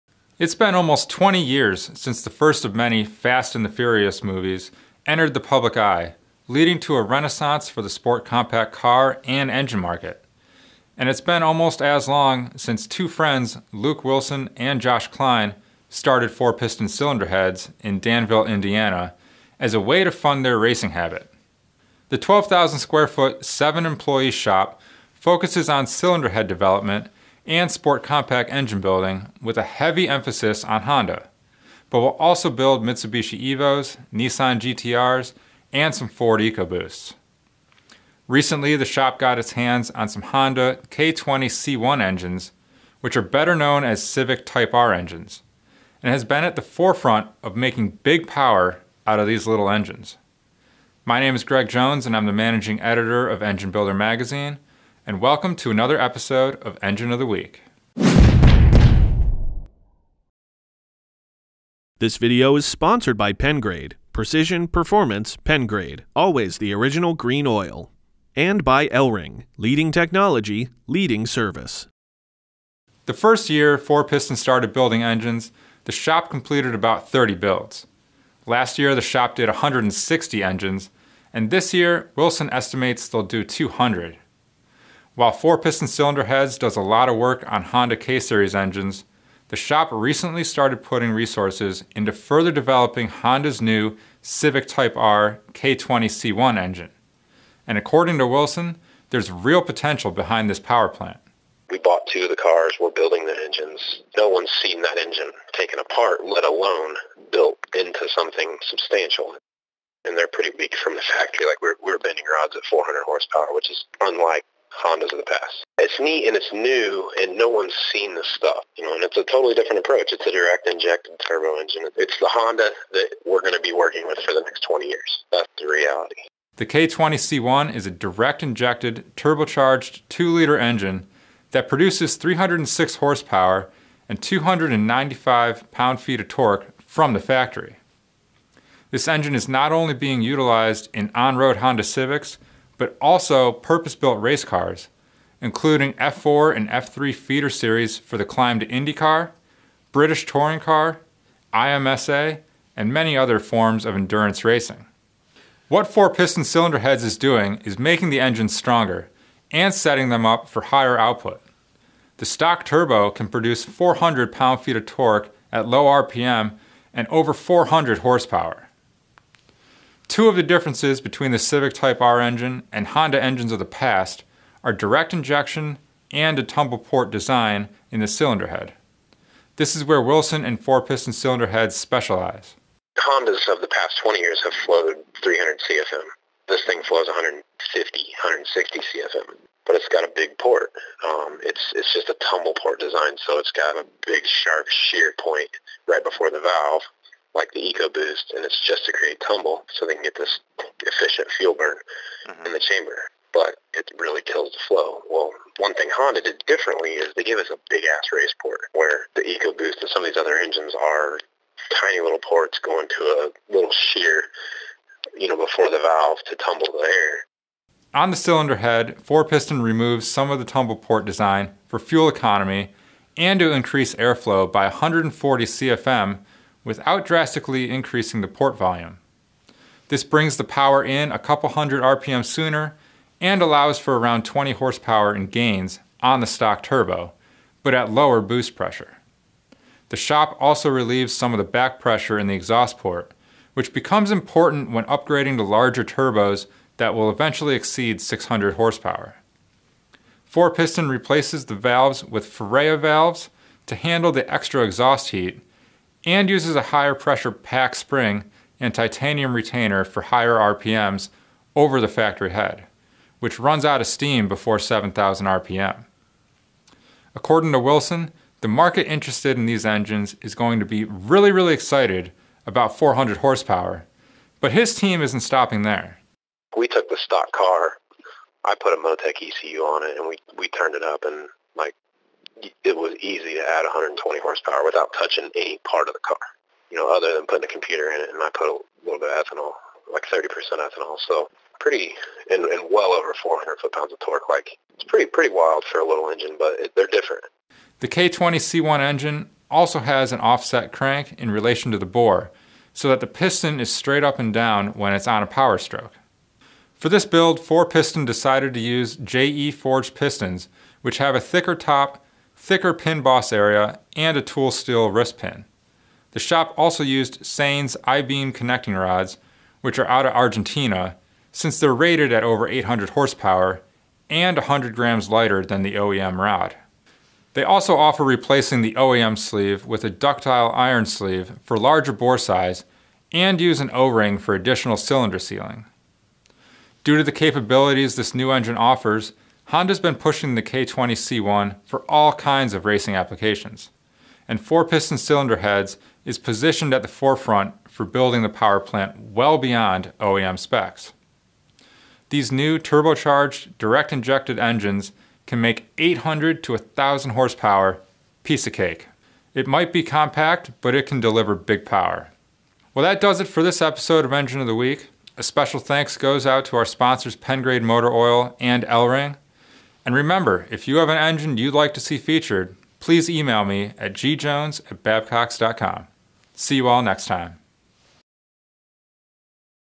Honda Civic Type R K20C1 Engine
This week we take an in-depth look at Honda's new Civic Type R engine, also known as the K20C1 engine. This particular build was done by the guys at 4 Piston Cylinder Heads in Danville, IN.